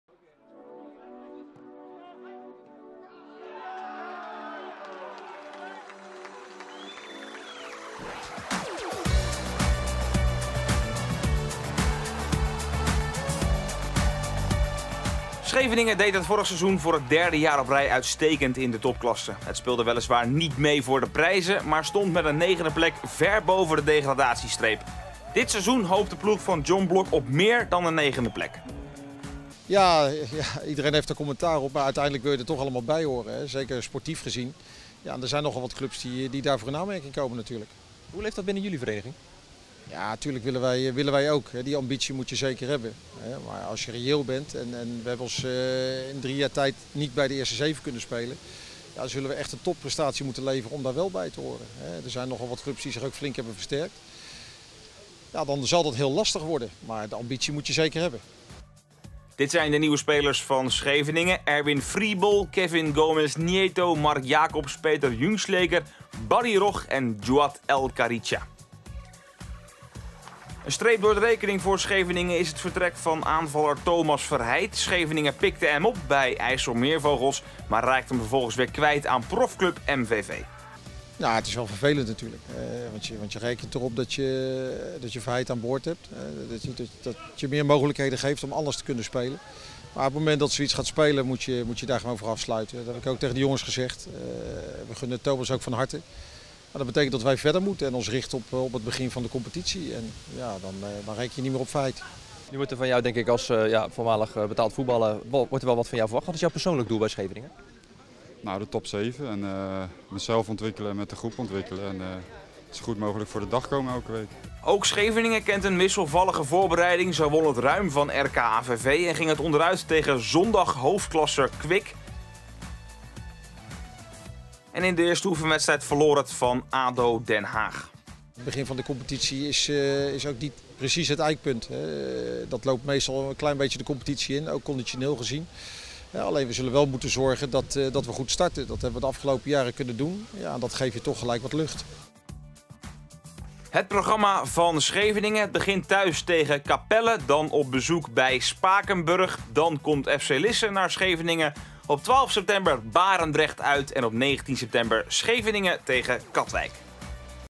Voorbeschouwing topklasse 2015-2016: Scheveningen
Voorbeschouwing topklasse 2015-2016: Scheveningen. Met trainer John Blok en middenvelder Peter Jungschläger.